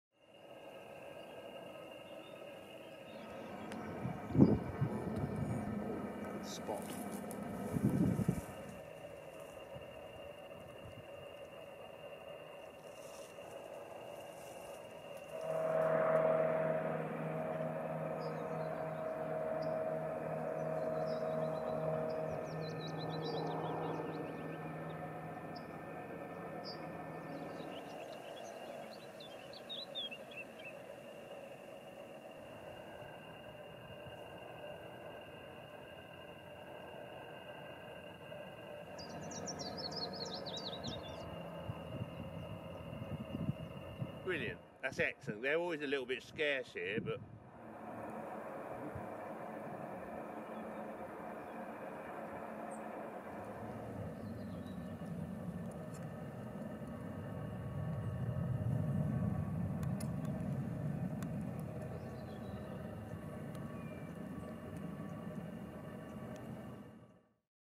Bison Hill butterflies
Butterflies seen on Bison Hill on the Dunstable Downs: Green hairstreak, Dingy and Grizzled Skippers and the Duke of Burgundy (including a mating pair)